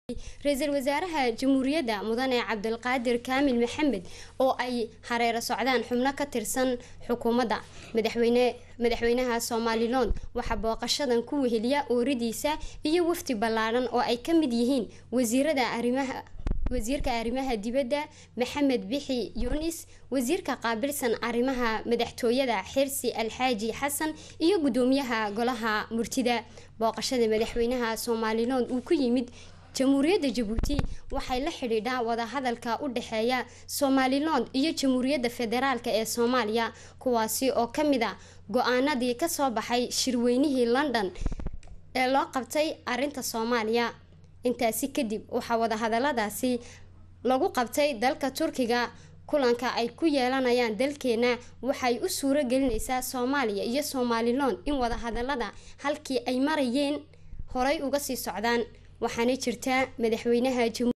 daawo sida loogu soo dhaweeyay wadanka jabuuti madaxwayne siilanyo.
wariye cabdinaasir cali ciidle